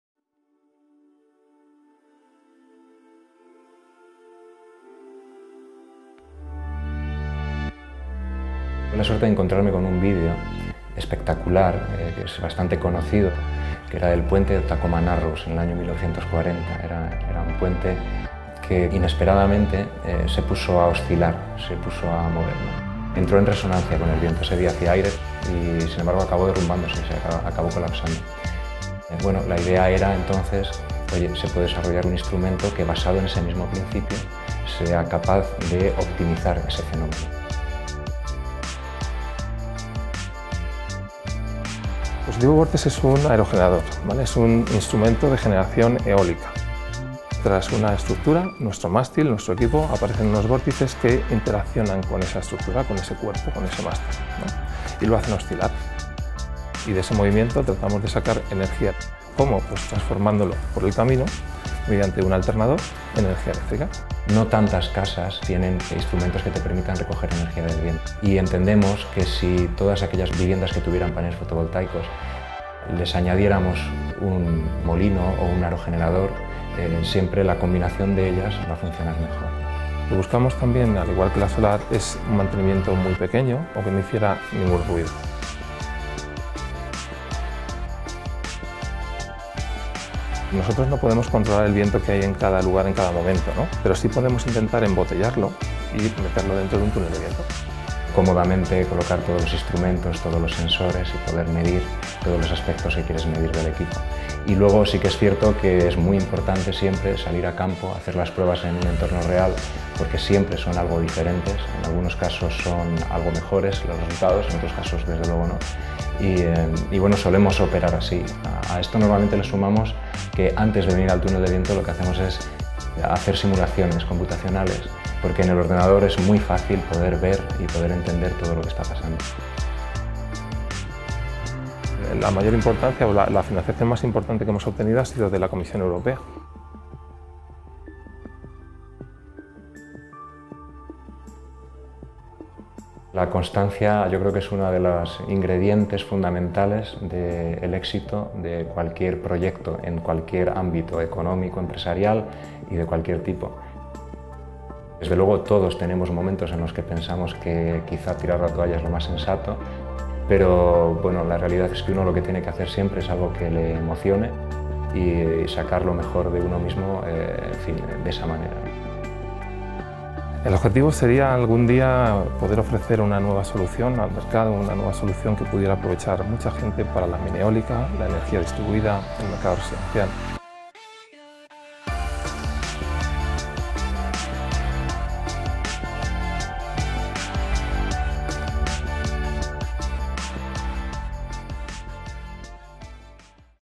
Bladeless wind turbines could be the future of Wind Power 🌬️ (2018)
Vortex Bladeless is a Spanish tech startup developing a new kind of wind turbine which needs no blades nor shafts. They are wind turbines that are not actually turbines since they don't rotate!

This new technology works by the Vortex Shedding phenomenon and brings exciting advantages versus regular wind power such as: low maintenance, silent, harmless to wildlife, low space requirements,  no lubricants needed, standalone and self-running.

Just plug your Vortex and get clean energy from the wind!

Vortex Bladeless, SL